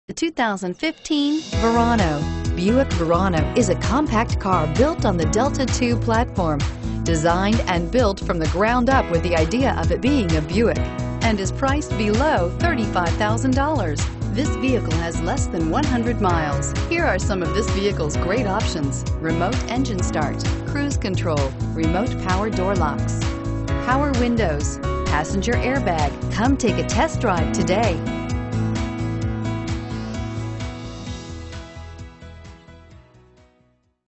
2015 Buick Verano Olds AB C5005
2015 Buick Verano Leather

Hildebrand Motors Ltd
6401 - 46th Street
Olds AB  T4H 1L7

Pick up this White 2015 Buick Verano, available today at Hildebrand Motors Ltd. Featuring Automatic transmission, it has 8 miles. This could be the one you've been searching for.